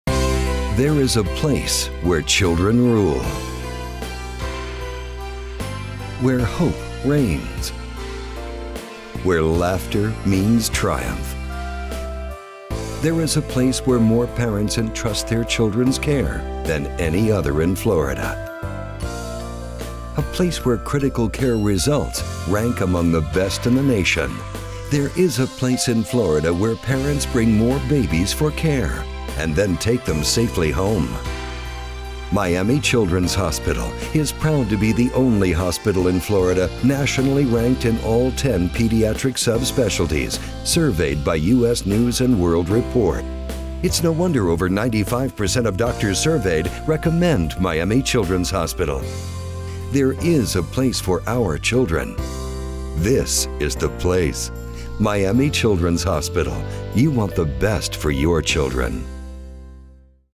" There's a place"Children's Hospital TV commercial, production company sample
This is one commercial of a three commercial campaign for Miami Children's Hospital . The spots were produced, directed, and edited by Neil Nunez of n2 Productions. They were shot on location and at Great Southern Studios by DP Jose Luis Ocejo using a Red One camera and an Optimo 24-290 lens. Original music composed and produced by Robert Morales. n2 Productions is a full service production company that specializes in producing TV commercials.